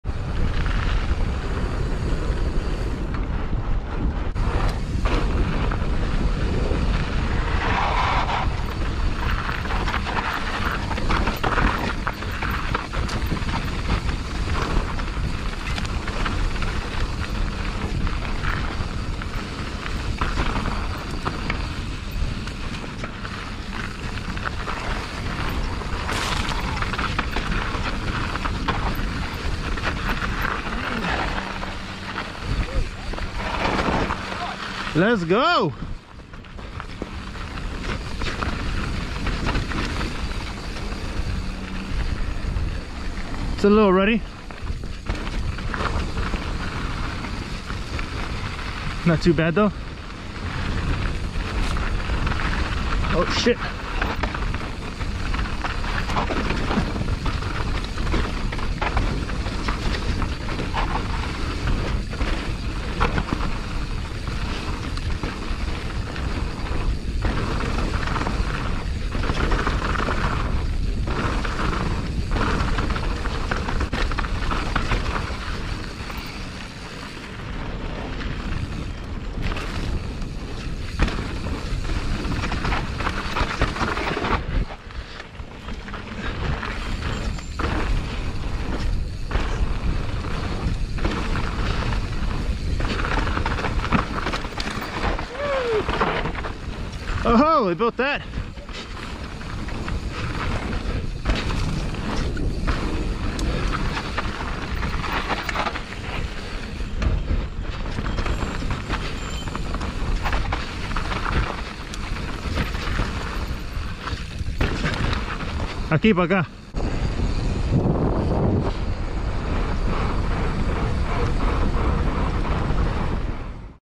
ShortSweetSeries // Horse Trail // Polygon Siskiu N7 // GoPro MTB 4K // Altadena CA
After Sunset Ridge you can pedal up and hit El Prieto, go straight and head out or go left and hit Horse Trail. That’s what we did! I took it slower due to the past rains and was unaware how the trail was after. But it was running great!